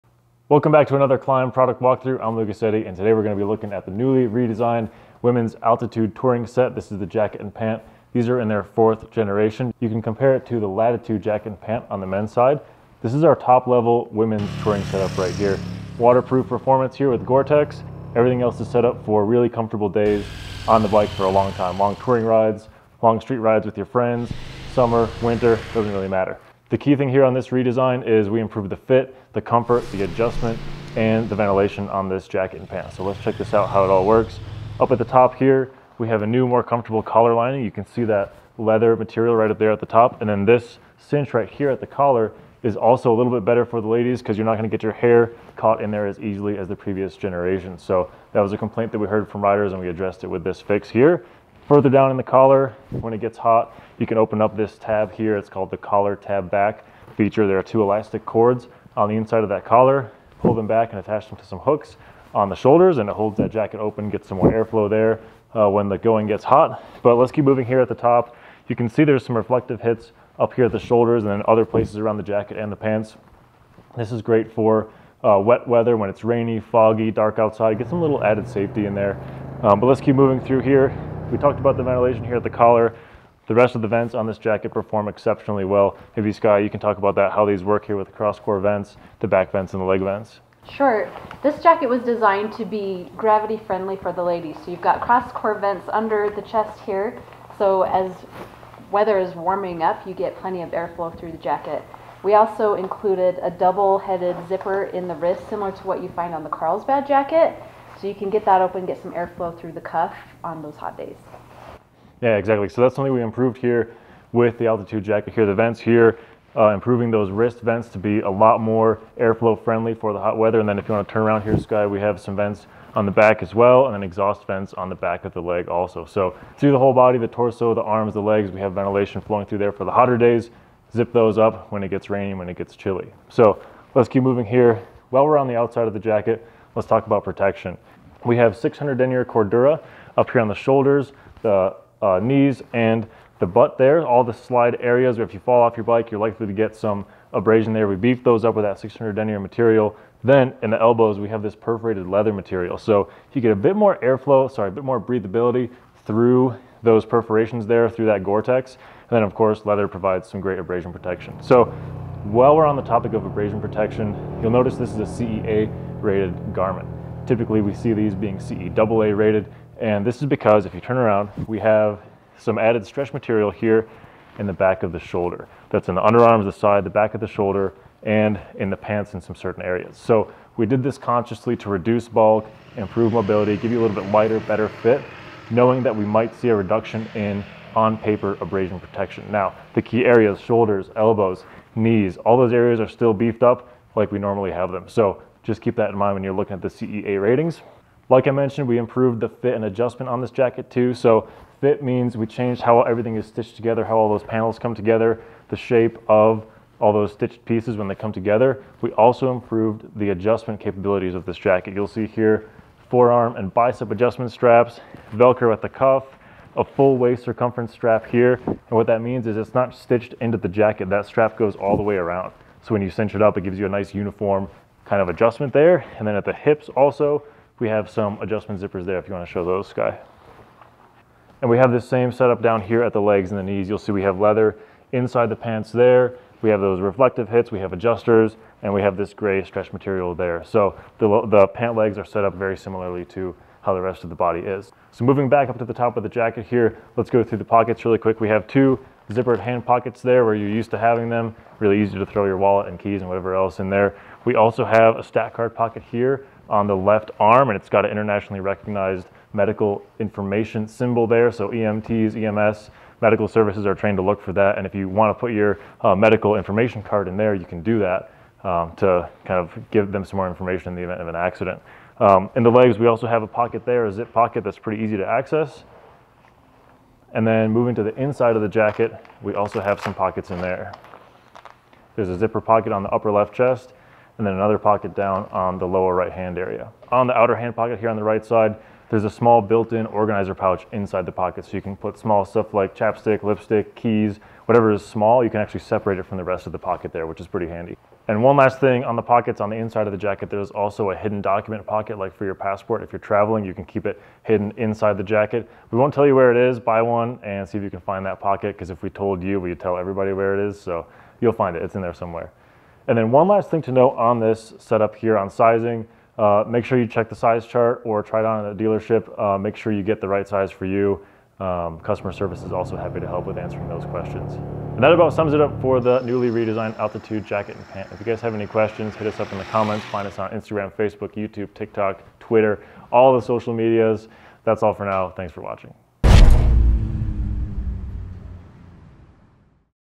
Altitude | Product Walkthrough
KLIM’s ever expanding suite of motorcycle gear features a redesigned Altitude Jacket for touring riders looking for the best in comfort, craftsmanship and style. Now in its fourth generation, the Women’s Altitude Jacket, now with reduced bulk, added stretch panels for mobility and new modernized styling, offers the perfect balance of touring function and riding features all certified to abrasion standard CE A EN 17092-4.

With a significant focus on overall fit, ventilation and storage,  the 2023 Altitude Jacket features an updated European-style fit, added GORE-TEX® stretch back and underarm gussets, new perforated goat leather elbows to enhance breathability and improved zippered vent layouts for better airflow. Complete with heavy-duty shoulder overlays, an adjustable shoulder and elbow CE Level 1 armor system with a Level 1 back pad, zippered hip gussets, a moisture-wicking antimicrobial breathable mesh liner and comfort trim, along with two cross-core chest vents, two back exhaust vents and two massive adjustable wrist vents with dual zippers to improve ventilation and make big, hot-weather rides even more comfortable.